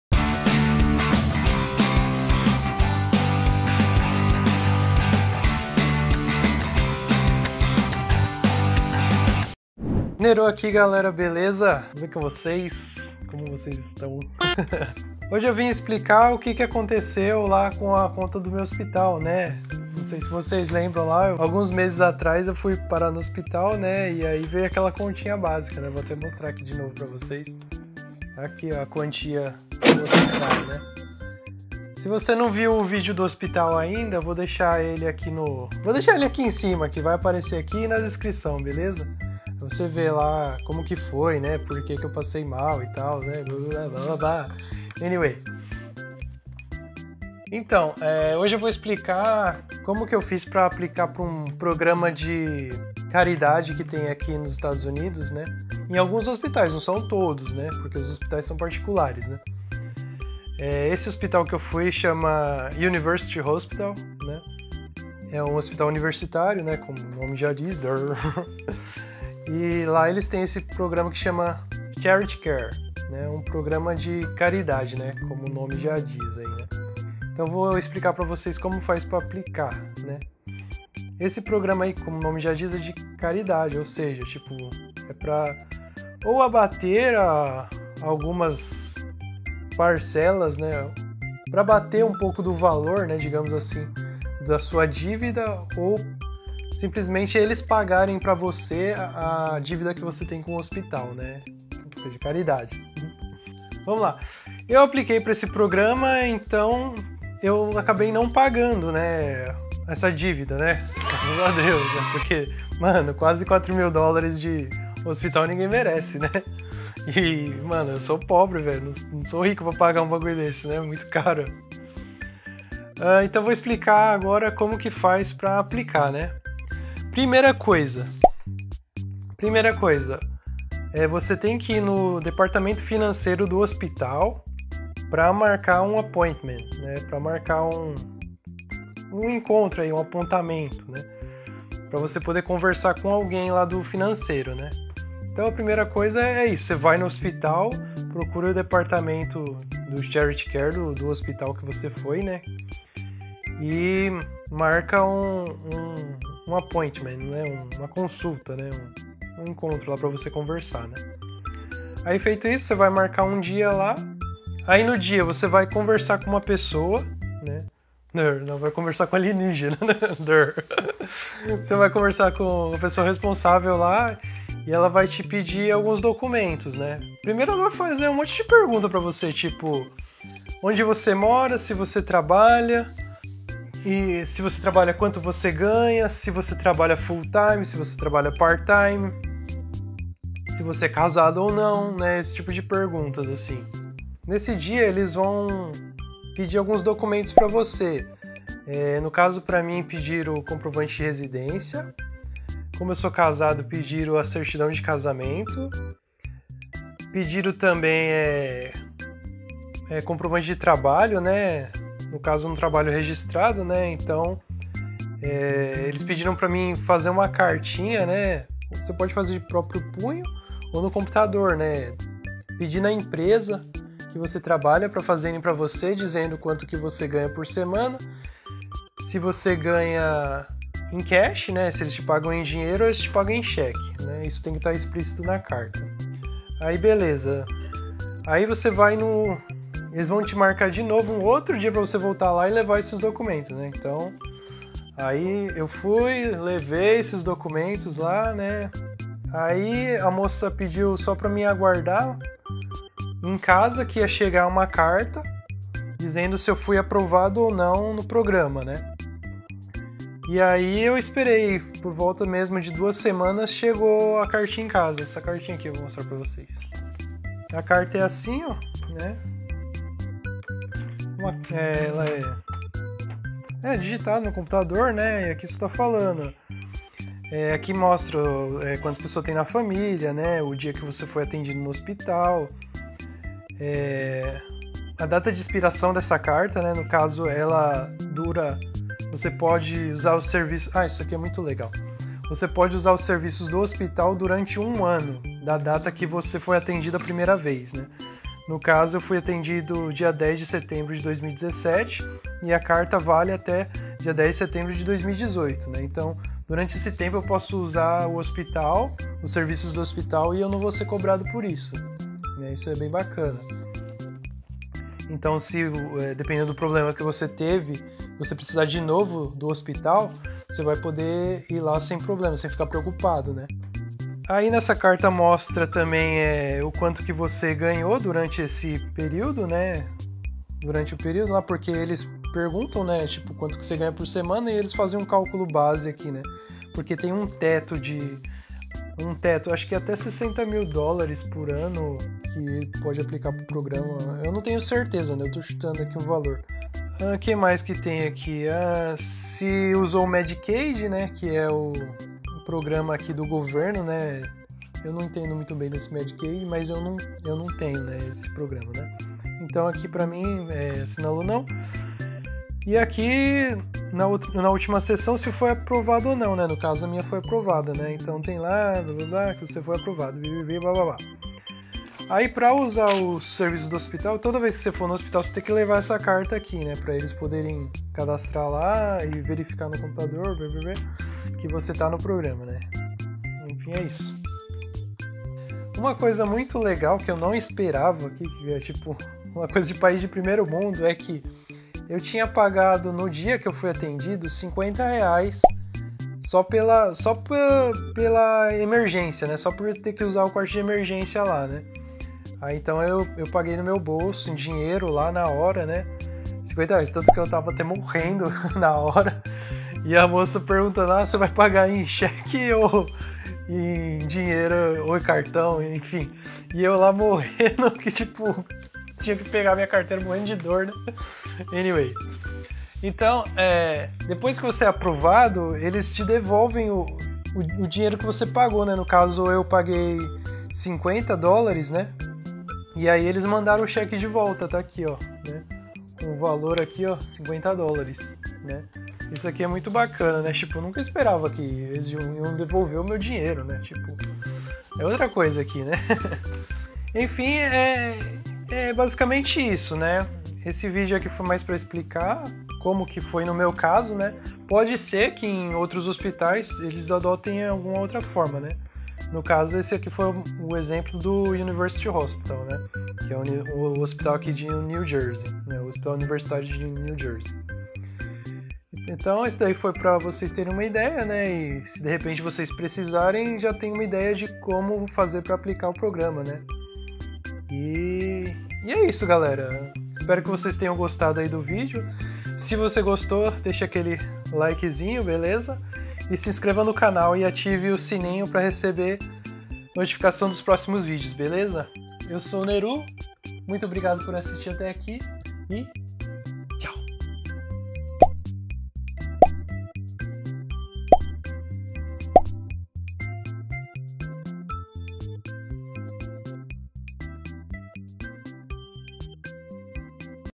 Hospital nos EUA - Aplicando pro Charity Care Program - Neru Meira - USA
Como fazer pra aplicar pro programa de caridade do Hospital.

tags adicionais: new patrick lopes turismo vlog estados unidos hospital